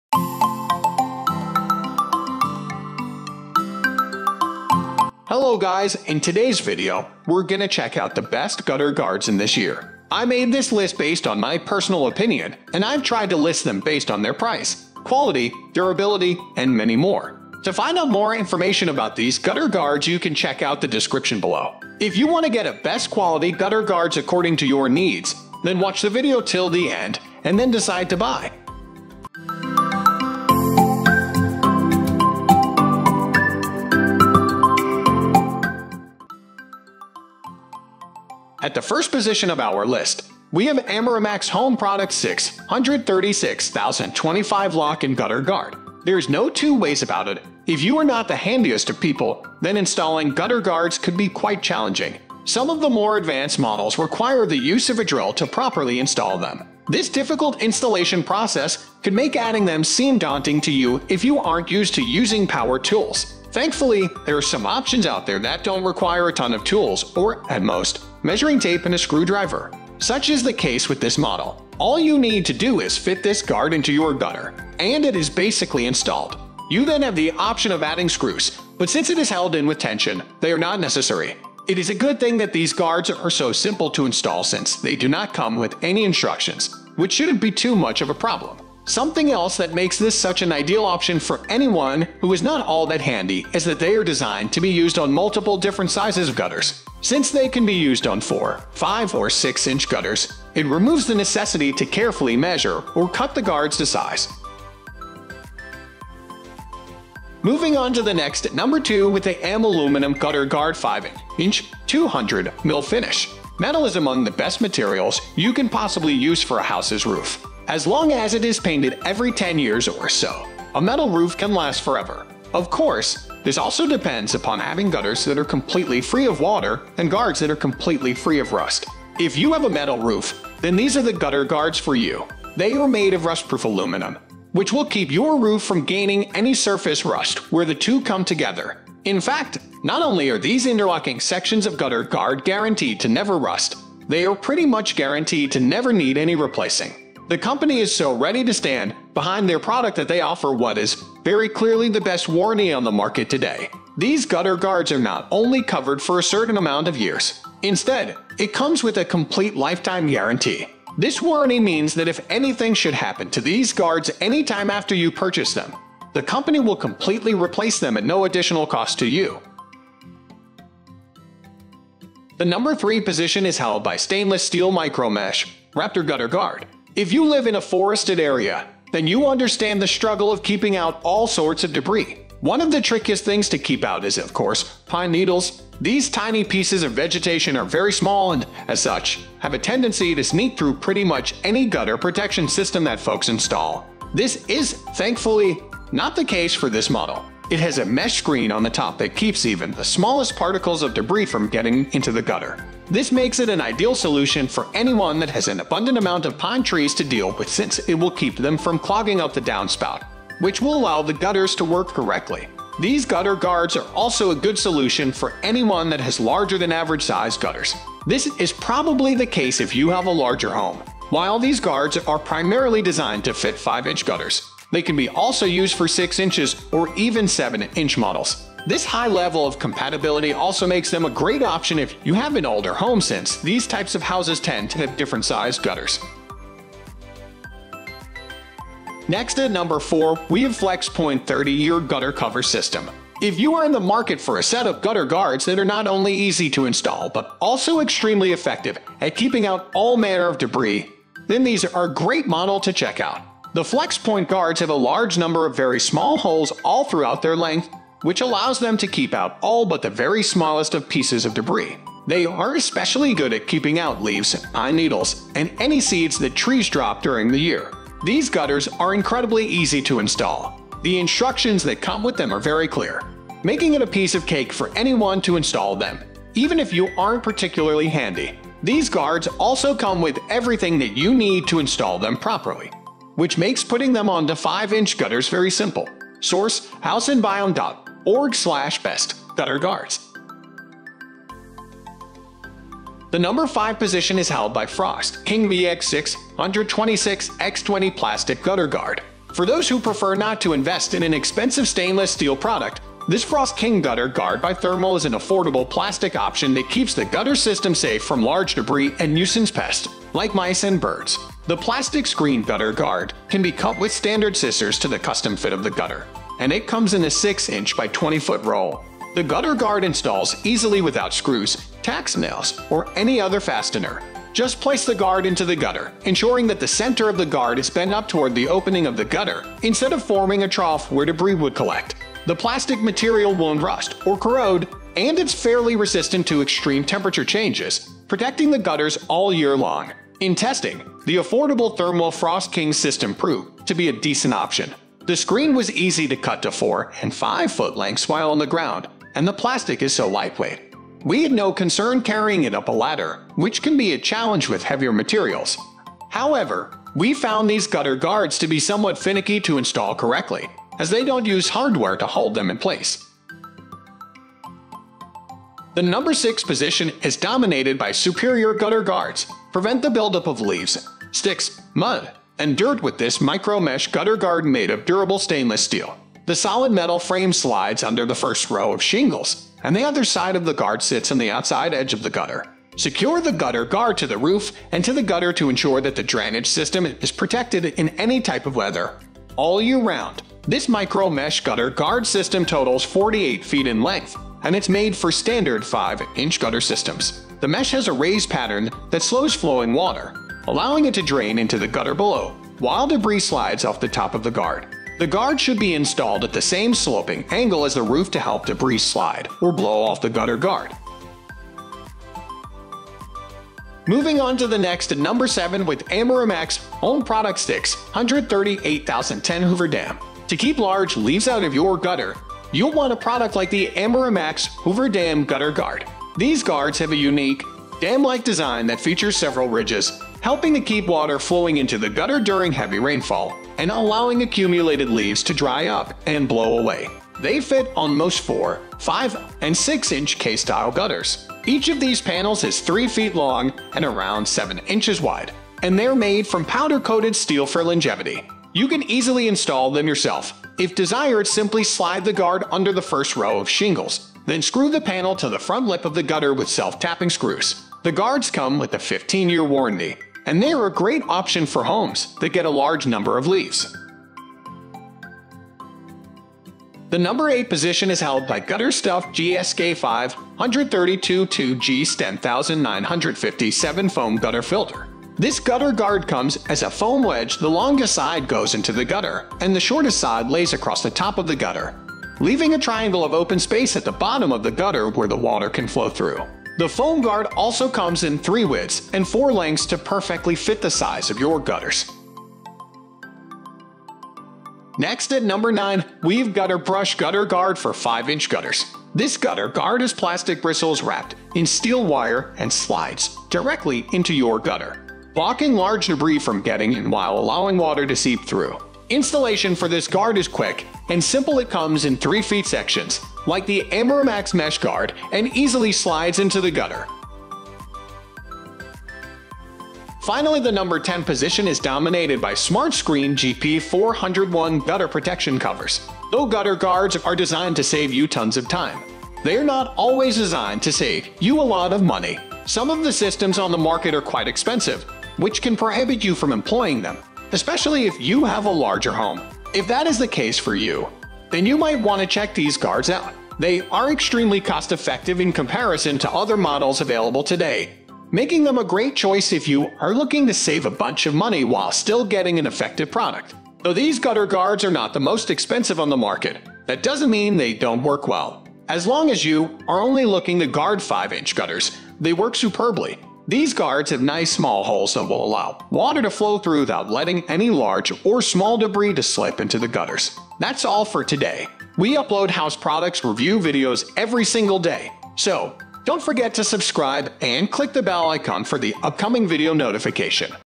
Top 10 Best Gutter Guards in 2024 | In-Depth Reviews & Buying Guide
Links to the Best Gutter Guards we listed in today's Gutter Guard review video:

1 . Amerimax Home Products 636025 Lock-In Gutter Guard
2 . A-M Aluminum Gutter Guard 5 inch (200', Mill Finish)
3 . Stainless Steel Micro-Mesh, Raptor Gutter Guard
4 . FlexxPoint 30 Year Gutter Cover System
5 . Frost King VX620 6"x20' Plastic Gutter Guard
6 . Superior Gutter Guards
7 . Amerimax Home Products 638010 Hoover Dam
8 . GutterStuff GSK532-2 GS10957 Foam Gutter Filter
9 . GutterBrush Gutter Guard for 5 Inch Gutters
10 . Smart Screen GPS401 Gutter Protection Covers,

What are the Best Gutter Guards in 2024?

In today's video we reviewed the Top 10 Best Gutter Guards on the market in 2024. We made this list based on our personal opinion and we ranked them in no particular order, after doing our research based on their prices, quality, durability, brand reputation and many more.

After watching this video you will know which are the best budget, top selling and top rated Gutter Guard on the market today.

If you choose from this list you can be sure that you buy one of the best Gutter Guards available today.

Portions of footage found in this video are not original content produced by the Homify team. Portions of stock footage of products were gathered from multiple sources including manufacturers, fellow creators and various other sources.